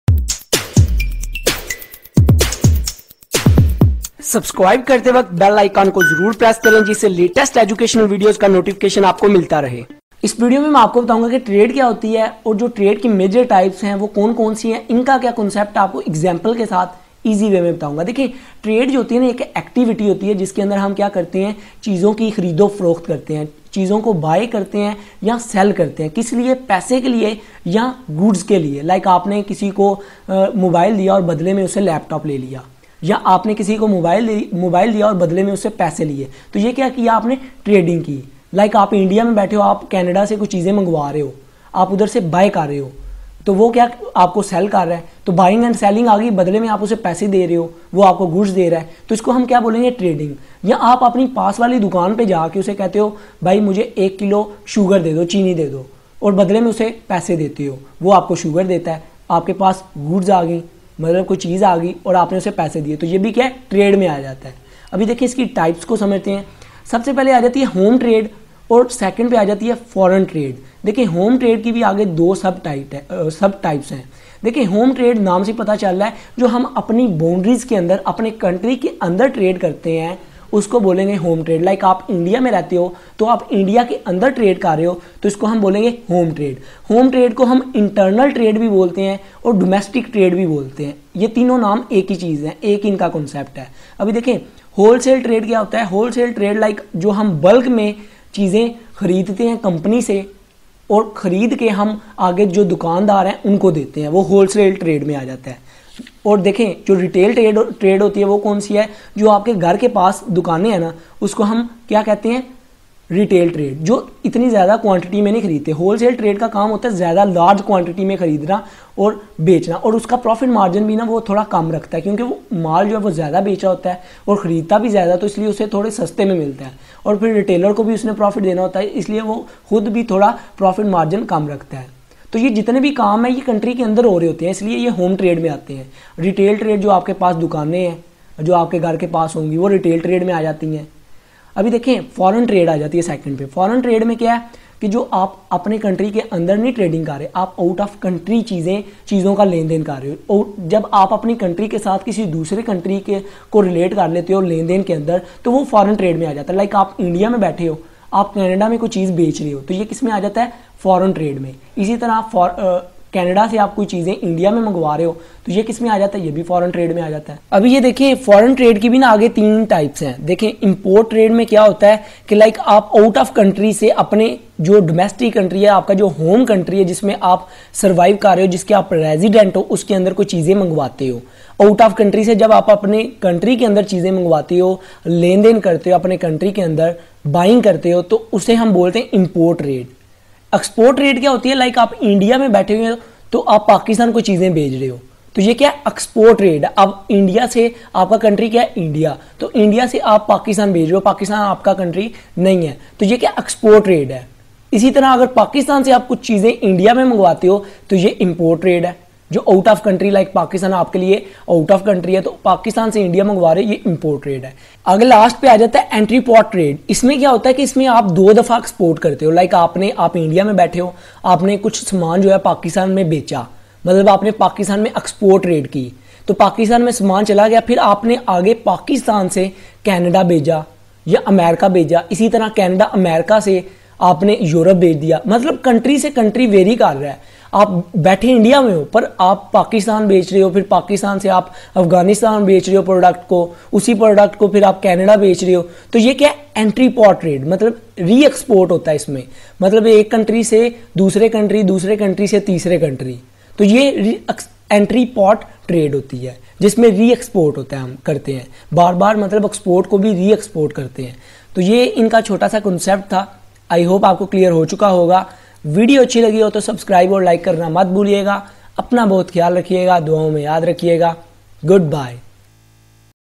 What is Trade & Types of Trade ? Urdu / Hindi
This Video Give the Concept of What is Trade & Types of Trade ? Urdu / Hindi
Following Types of Trading:
Home Trade
Internal Trade
Domestic Trade
External Trade
Wholesale Trade
Retail Trade
Foreign Trade
Import Trade
Export Trade
Entrepot Trade